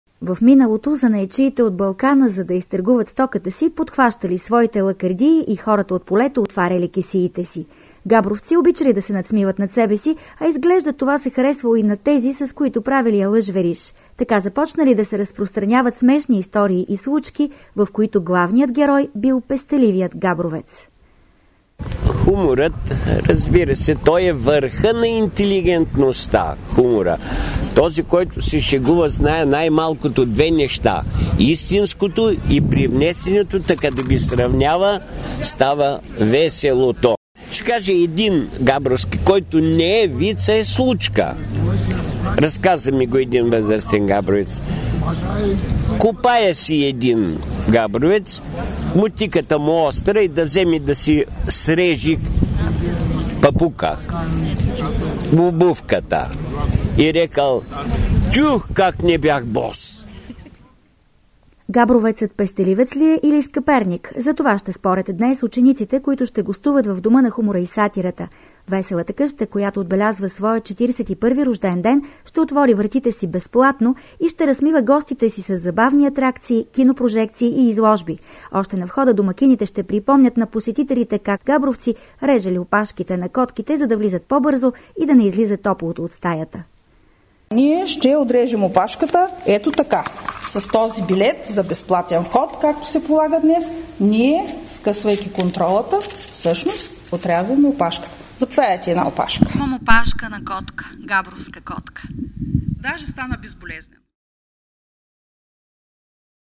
1-ви април международния ден на хумора и шегата
Днес габровци празнуват международния ден на хумора и шегата.
1 ви април е на особена почит в града на смеха, чийто жители са прочути с анекдотите и веселите истории, които разказват за самите себе си.